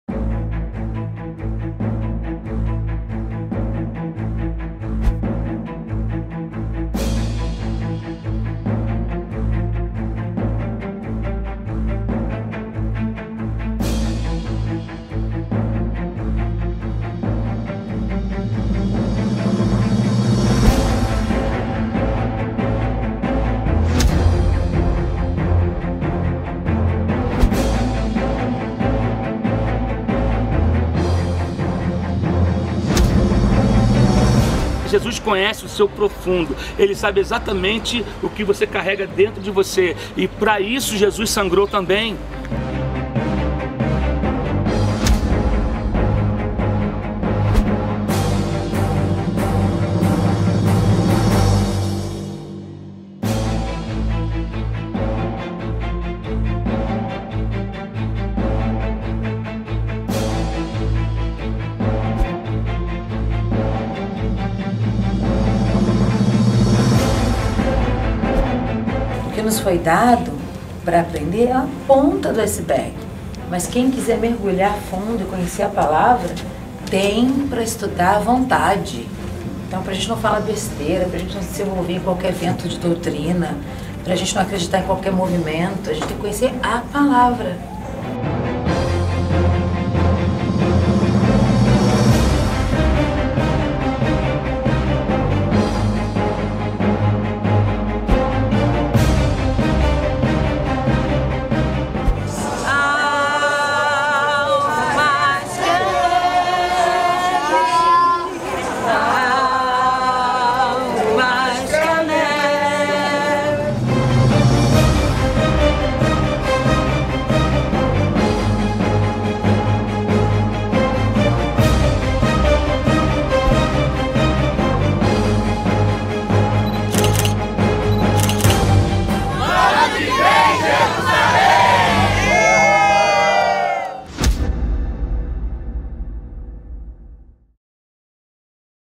Fernanda Brum em Israel | Episódio 1
Fernanda Brum em uma série especial sobre a Caravana 2020 em Israel, este é o episódio 01. Siga, Curta e Compartilhe com todos os seus amigos e irmãos em Cristo.

▸ Gestão Canal: Agência Petra